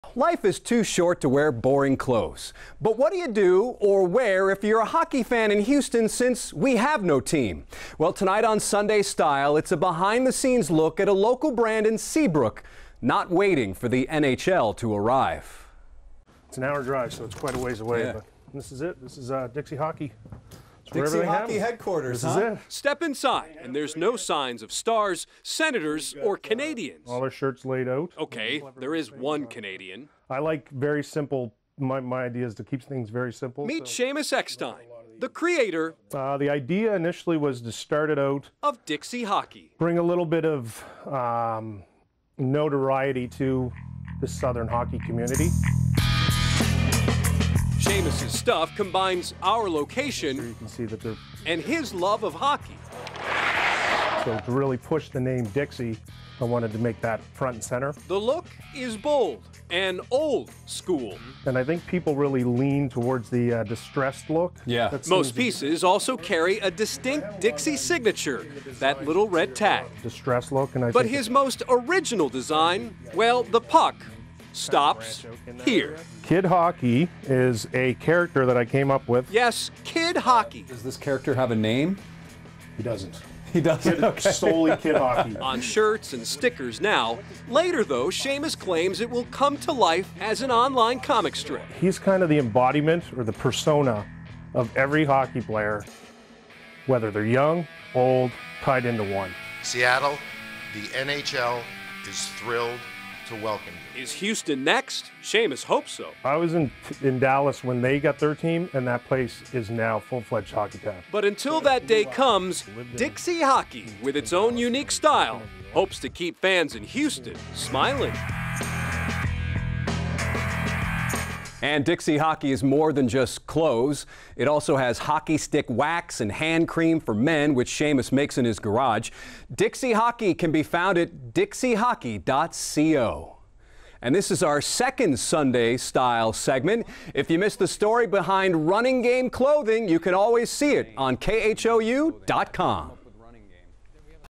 Sports Extra: Dixie Hockey in Seabrook bringing notoriety to southern hockey community
Life is too short to wear boring clothes. But what do you wear if you're a hockey fan in Houston, since we have no team? So we take a behind-the-scenes look at a local brand in Seabrook not waiting for the NHL to arrive.